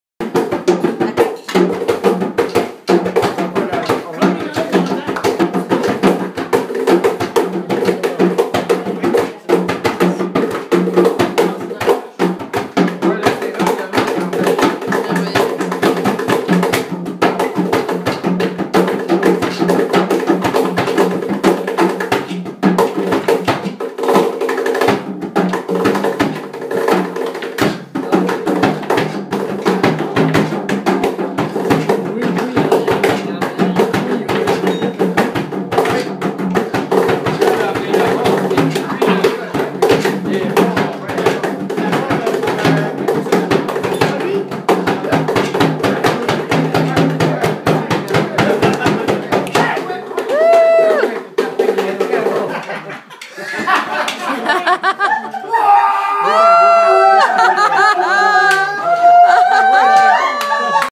jam25dec2015
Petit jam avec Louis (cajon) et Roch (jembé) Lévesque
Noel 2015